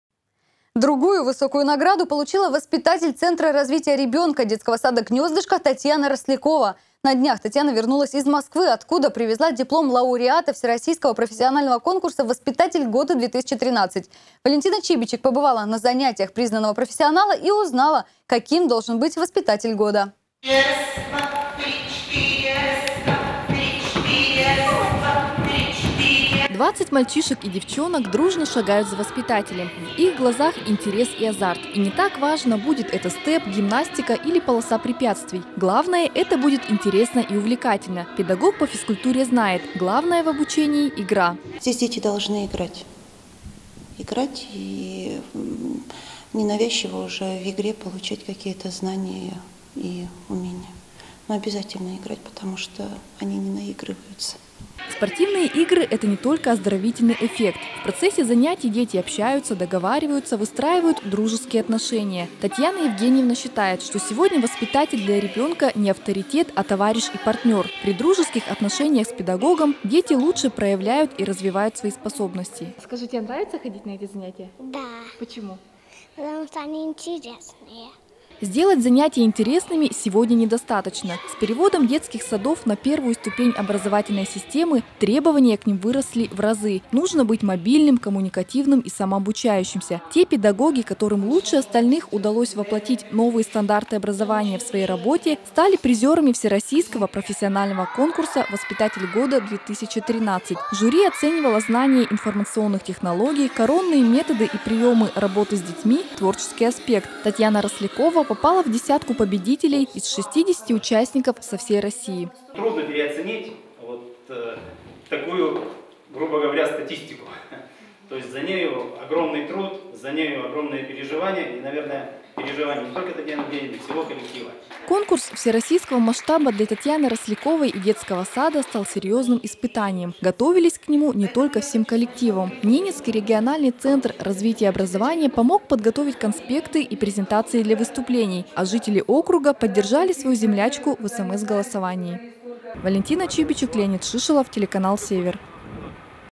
Воспитатель из НАО стала лауреатом конкурса «Воспитатель года -- 2013»
Высокую награду получила воспитатель Центра развития ребенка — детского сада «Гнездышко» Татьяна Рослякова. На днях Татьяна вернулась из Москвы, откуда привезла диплом лауреата Всероссийского профессионального конкурса «Воспитатель года -- 2013».